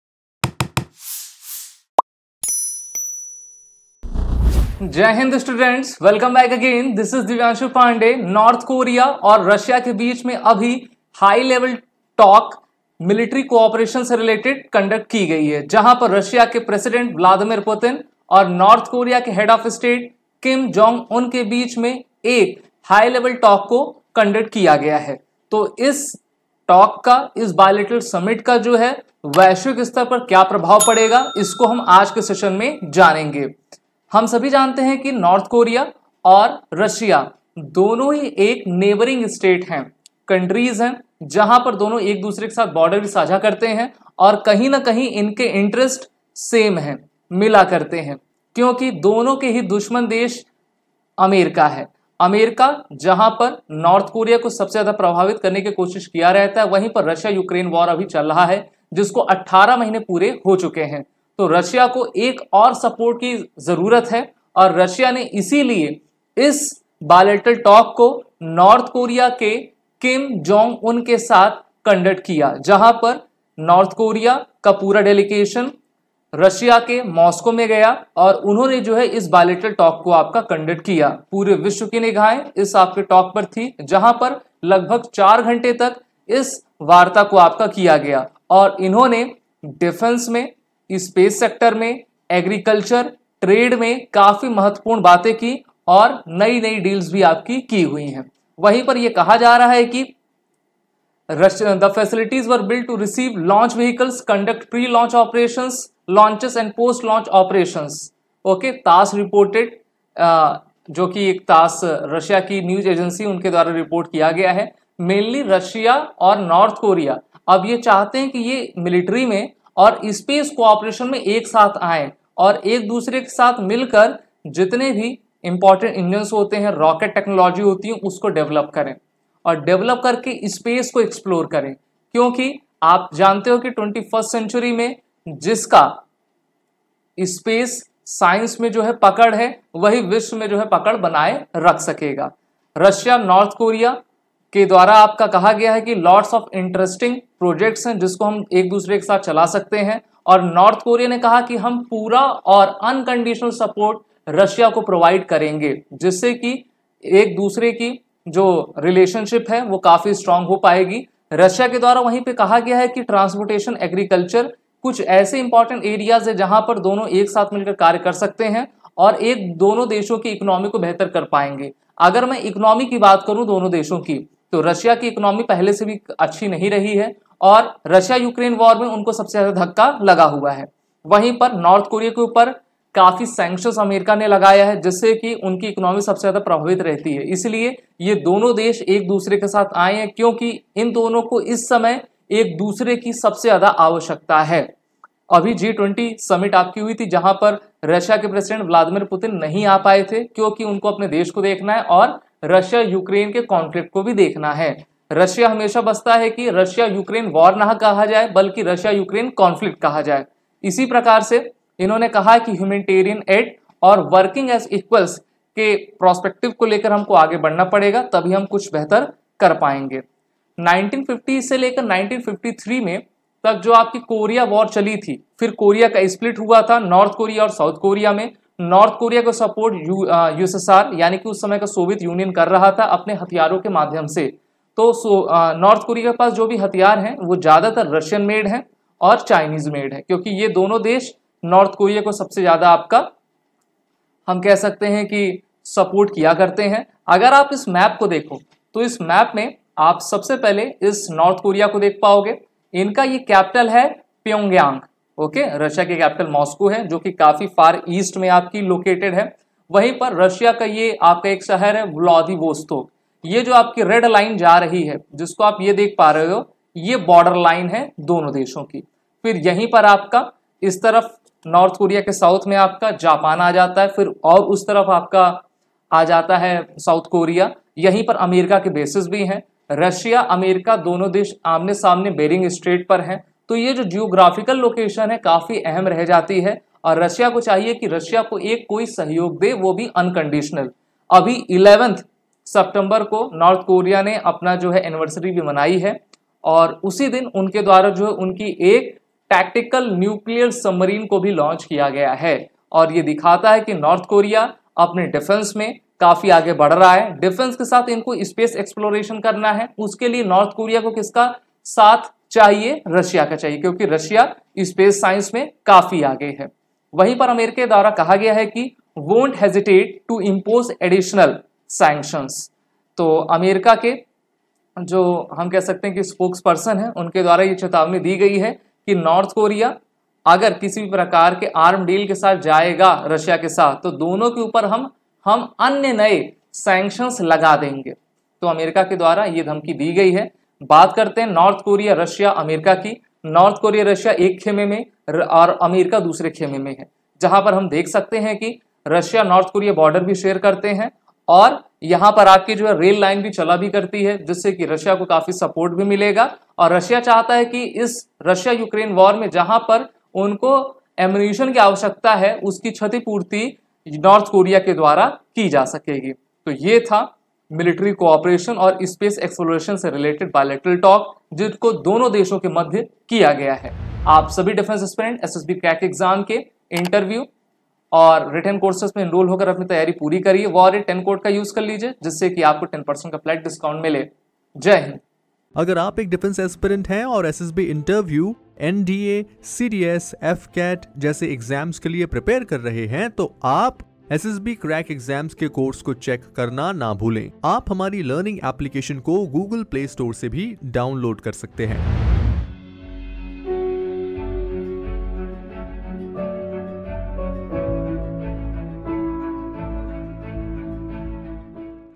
'Full & Unconditional Support' Kim to Putin | Russia & North Korea Bilateral Meet | SSB Interview
North Korea's Kim Jong Un vowed "full and unconditional support" for Russia's Vladimir Putin on Wednesday as the two leaders isolated by the West held a summit that the U.S. warned could lead to a deal to supply ammunition for Moscow's war in Ukraine.